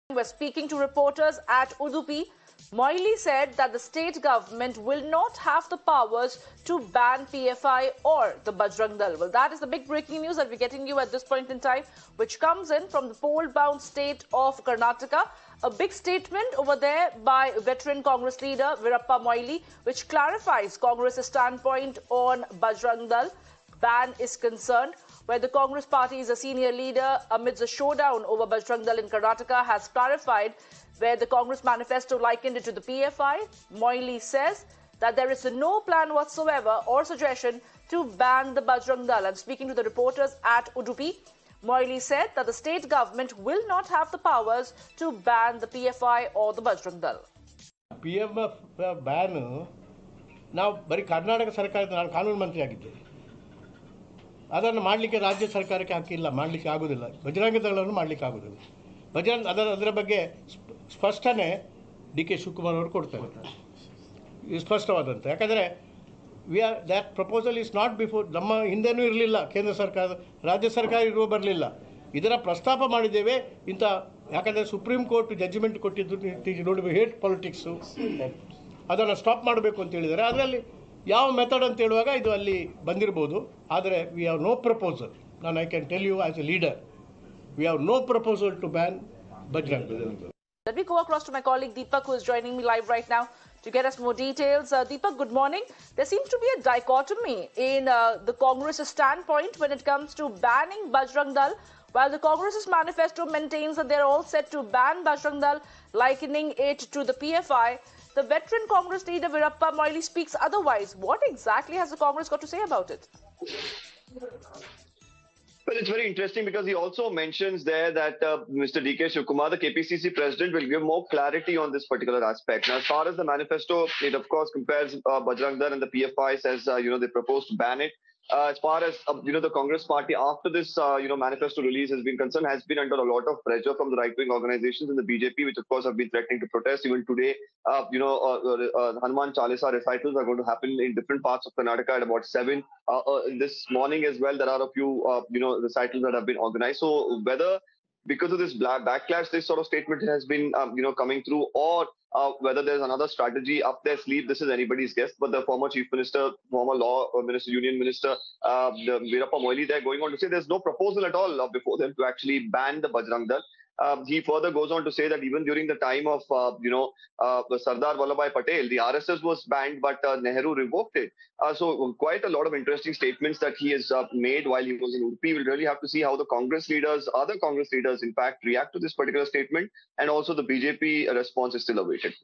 Karnataka polls 2023: 'State can't ban Bajrang Dal', Congress leader Veerappa Moily clarifies
Speaking to reports at Udupi Veerappa Moily said state government will not have the powers to ban PFI or the Bajrang Dal. Congress election manifesto for Karnataka has promised to ban organisations such as Bajrang Dal and Popular Front of India for, "promoting enmity or hatred, whether among majority or minority communities," after BJP leaders attacked the promise to ban Bajrang Dal.